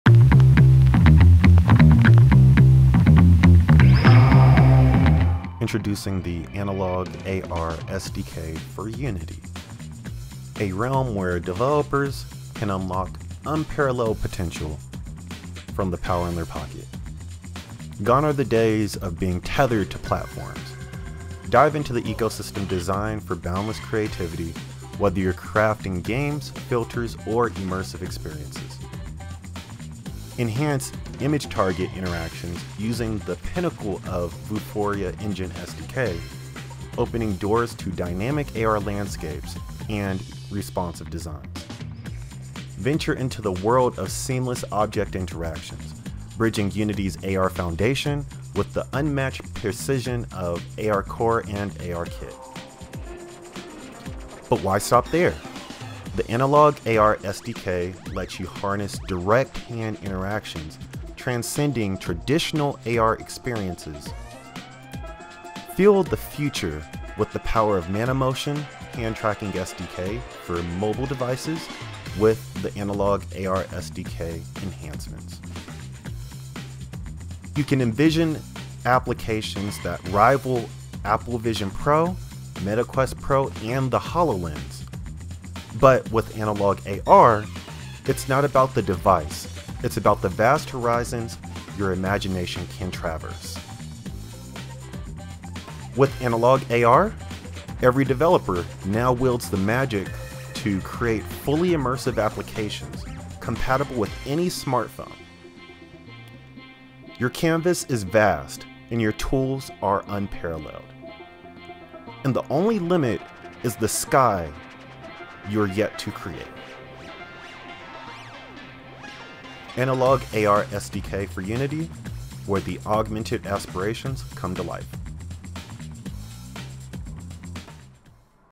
Analog AR Augmented Reality SDK for Unity
ANALOG-AR Augmented Reality headset provides a low-cost solution for consumers to develop and experience Augmented Reality and Mixed Reality content for mobile devices. This is an affordable alternative to traditional head mounted displays that reduces the cost to engage in hands free XR experiences to a fraction. This solutions is application and device agnostic. It leverages the power of current and future mobile technology such as processors and sensors, and frees the hands to have a natural immersive experience. This includes mixed reality, hand tracking, and augmented books. Simply, attach the phone to the front, open up your favorite app, put on the headset, and enjoy.